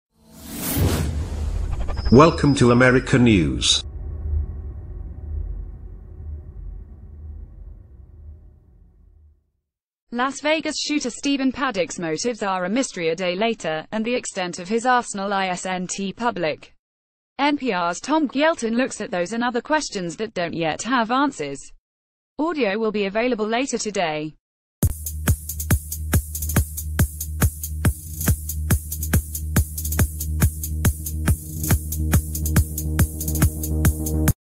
America News - What we don't yet know about the las vegas attacks
Las Vegas shooter Stephen Paddocks motives are a mystery a day later, and the extent of his arsenal isnt public. NPRs Tom Gjelten looks at those and other questions that dont yet have answers. Audio w...